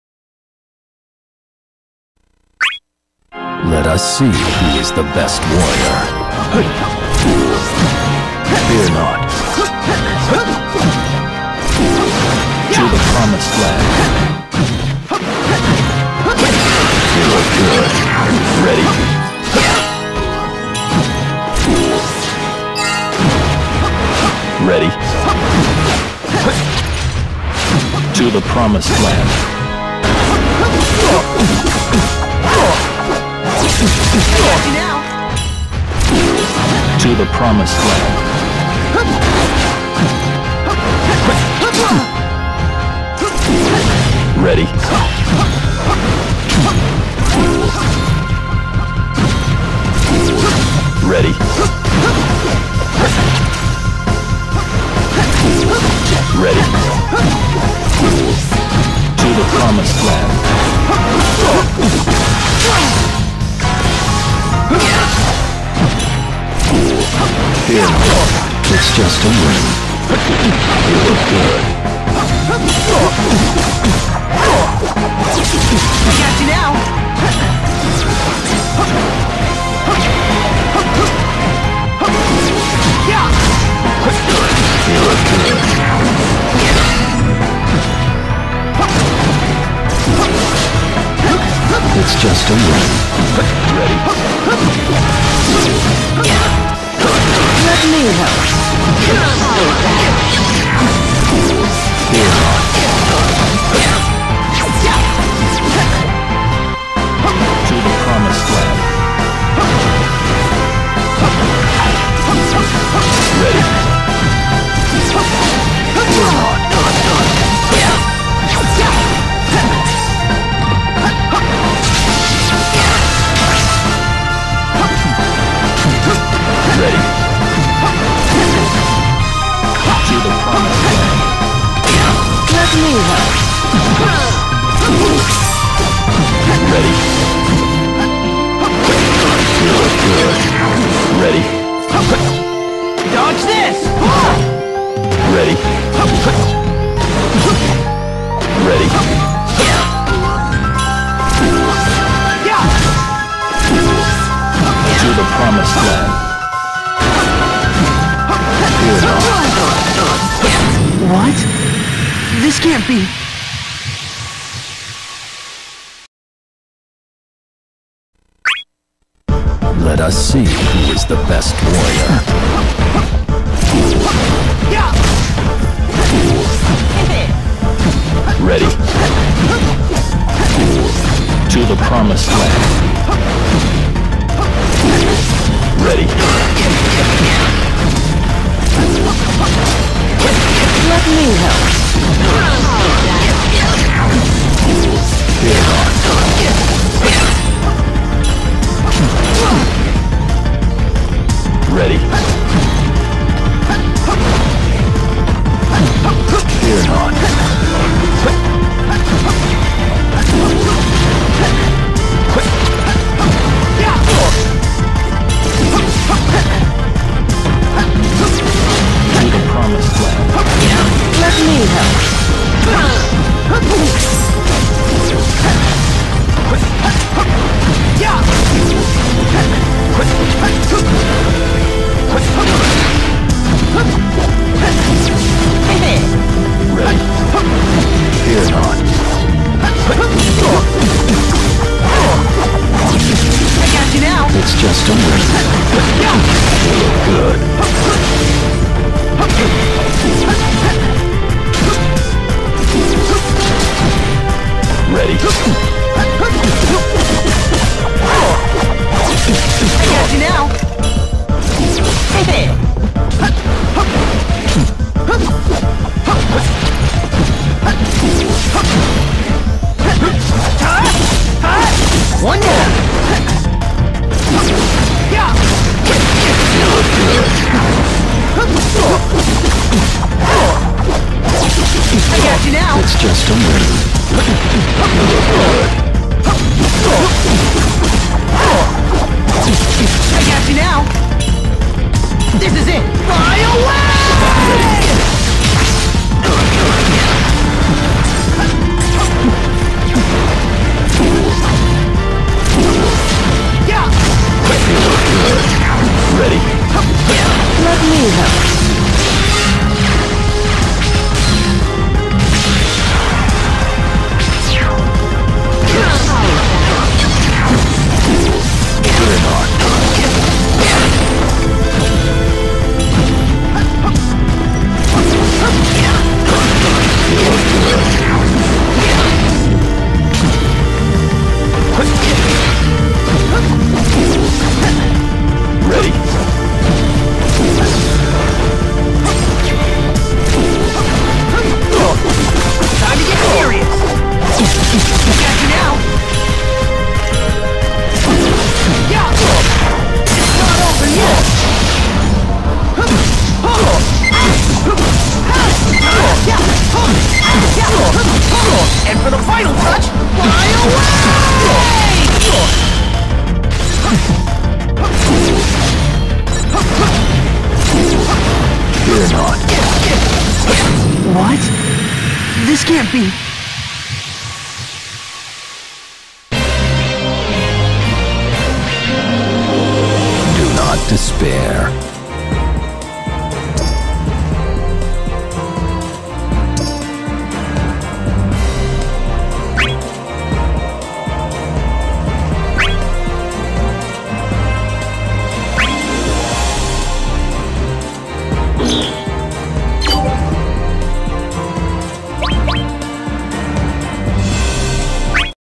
[LevelUp R1] Barder (KH Sephiroth) vs Sparki (Zidane) - M1 & M2
[Matches Start Time: Match 1 @ 0:03 and Match 2 @ 2:48]
[Get Your Game On Series - LevelUp Round 1] Barder (KH Sephiroth)  vs Sparki (Zidane) - M1 & M2

Connectivity: ?/5 {?}